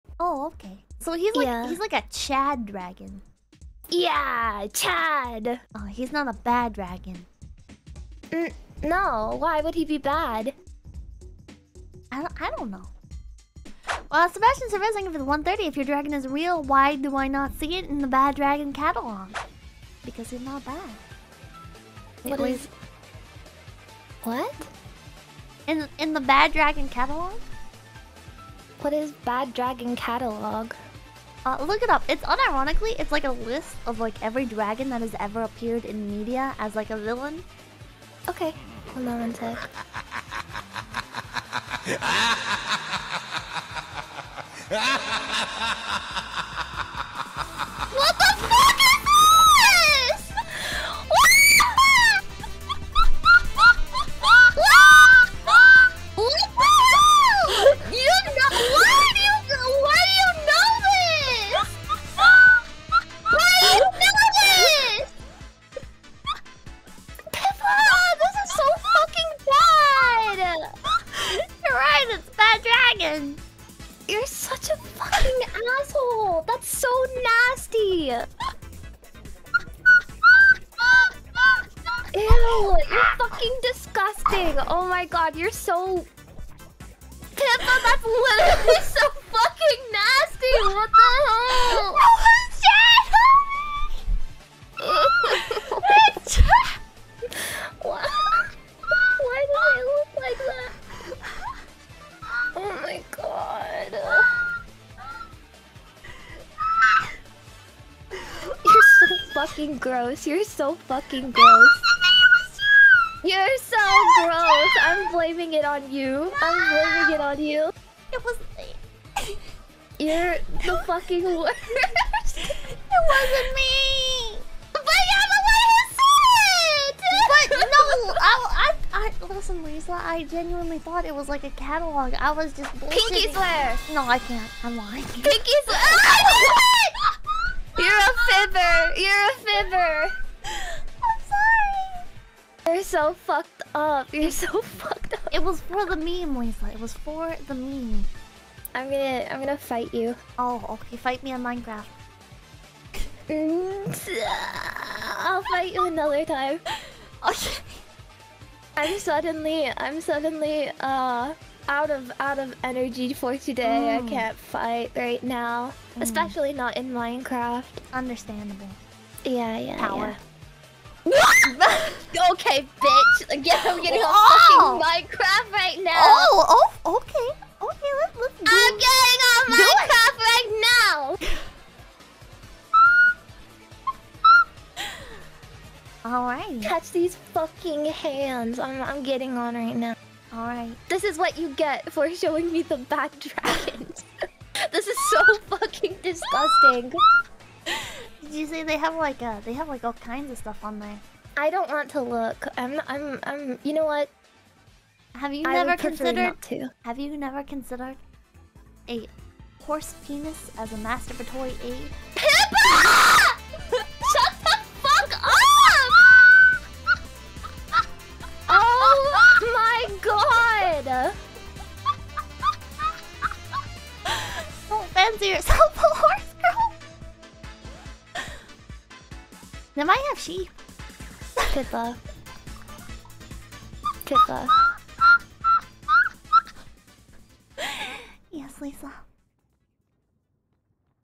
Bad Dragon Catalog [Pipkin Pippa and Chikafuji Lisa Classics]
One of my favourite Pippa x Lisa clips and usually the first one I show new Pippa fans. It's the Bad Dragon Catalog collab, where Pippa tricks Lisa into searching "Bad Dragon"

Much innocence was lost that day. Something was awakened and The Nightmare Sheep was never the same.

Find the Vtubers here:

Pippa: @PipkinPippa
Lisa: @chikafujilisa